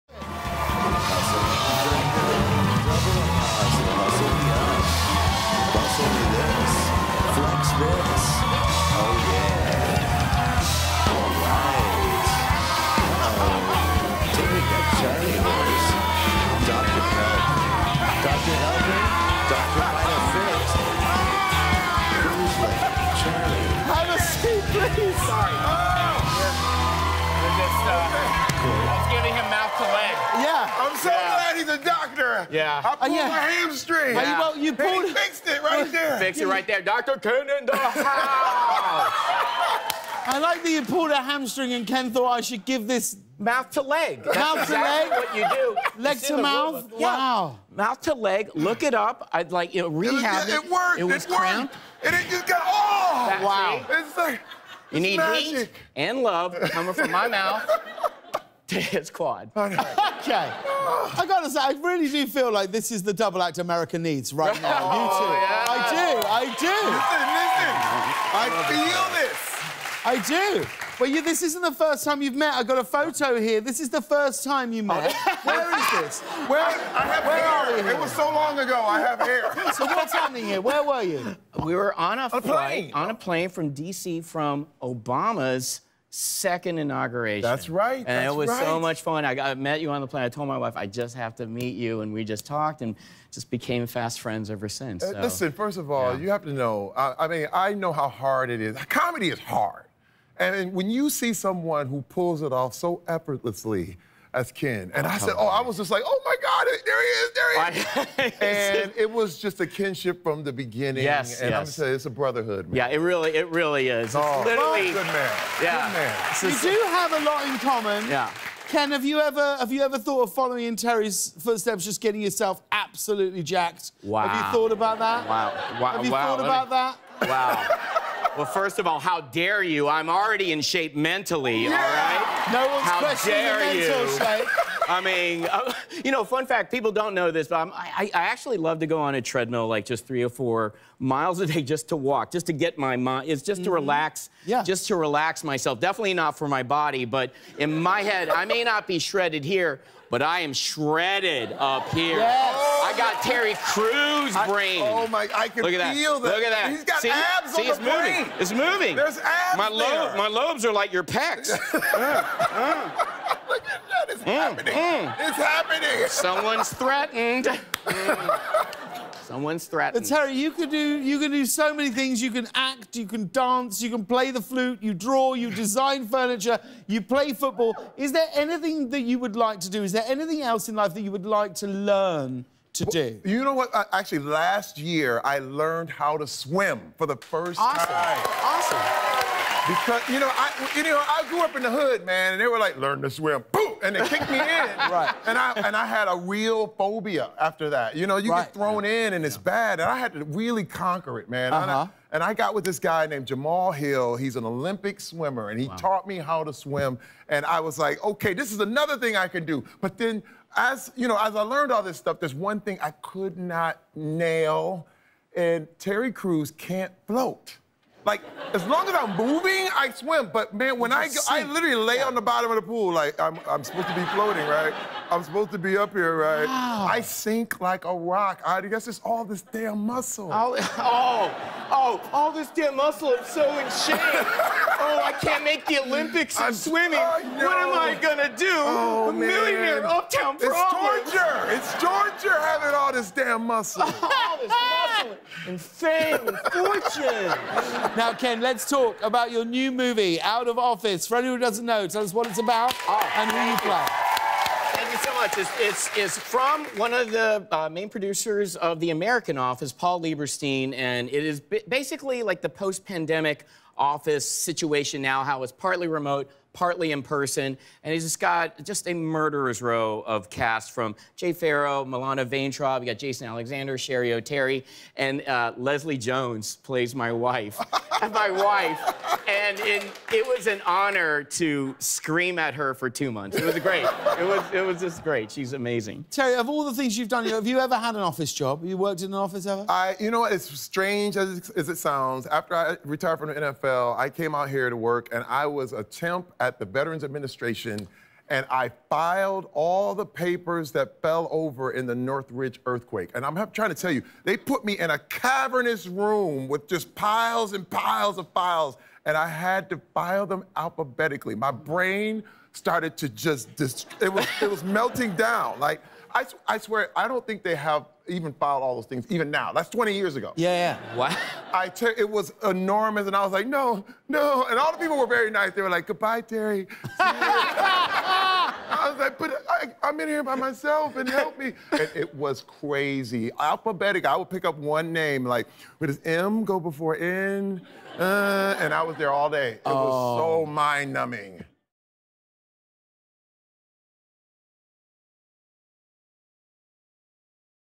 Dr. Ken Jeong Is In The House!
James welcomes guests Terry Cruz and Ken Jeong, the buddy comedy duo America needs. Terry and Ken walk James through their first meeting, and first selfie. Terry and Ken then compare their different strengths and weaknesses, including Terry's inability to float in water. And Ken describes his new movie "Out of Office," which reminds Terry of his time organizing paperwork in a mind-numbing office.